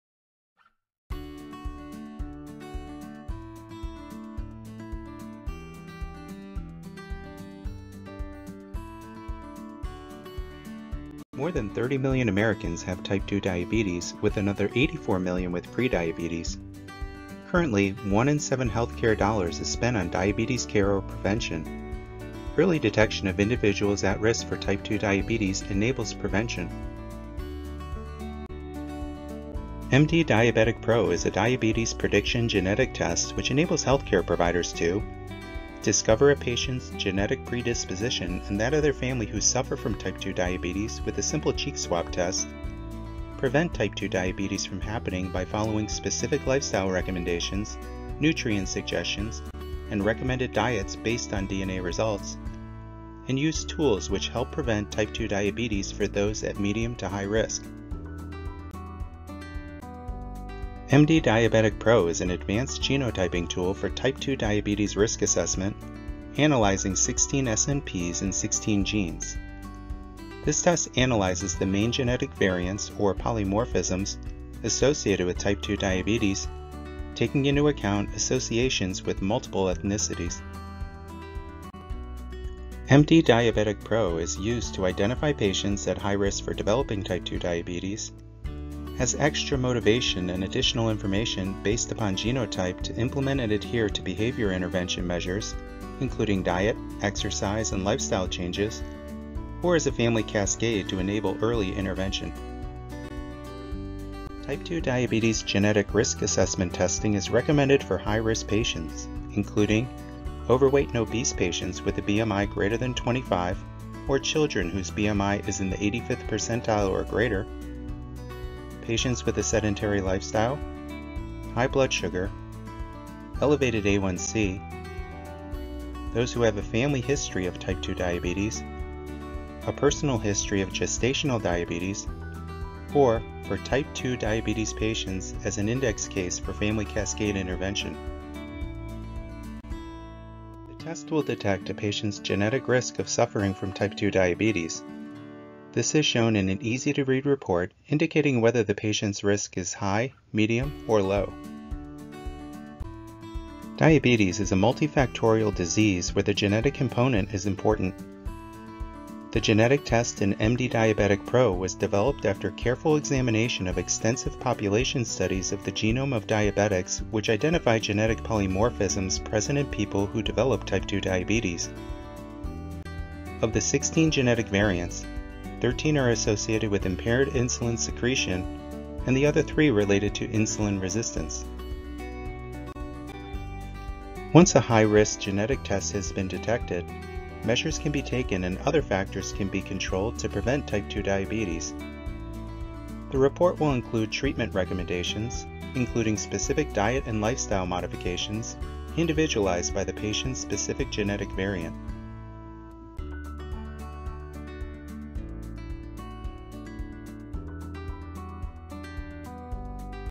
MD Diabetic Pro - Genetic Test for Type 2 Diabetes
More than 30 M Americans have type 2 diabetes, with another 84M with pre diabetes. Currently, 1 in 7 healthcare $ is spent on diabetes care or prevention. Early identification of individuals at risk for type 2 diabetes enables prevention.

MD DIABETIC PRO® IS A DIABETES PREDICTION GENETIC TEST WHICH ENABLES HCPS TO:
1) Discover a patient’s genetic
predisposition and that of their family who suffer from type 2 diabetes with a simple cheek swab test.
2) Prevent type 2 diabetes from happening by following specific lifestyle recommendations, nutrient suggestions and recommend diets based on DNA results.
3) and Use tools which help prevent of type 2 diabetes for those at medium or high risk.

To add this testing option to your practice, contact your Practice Development Manager.